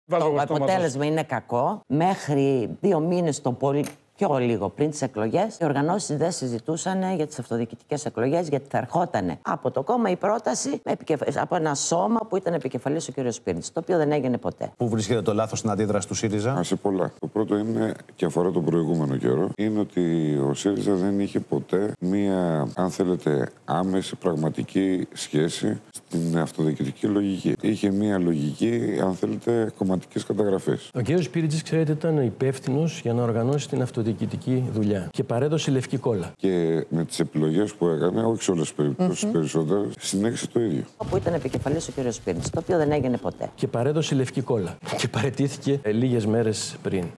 Πέρκα Σκουρλέτης "δείχνουν" Σπίρτζη υπεύθυνο για την πανωλεθρία ΣΥΡΙΖΑ στις αυτοδιοικητικές εκλογές
Τσίρκο.

Βγαίνει ο Σπίρτζης και κουνάει το δάχτυλο για την πανωλεθρία του ΣΥΡΙΖΑ στις αυτοδιοικητικές εκλογές και βγαίνουν Πέρκα - Σκουρλέτης και αποκαλύπτουν ότι ο Σπίρτζης ήταν υπεύθυνος για την κατάρτιση των ψηφοδελτίων και δεν έκανε τίποτα.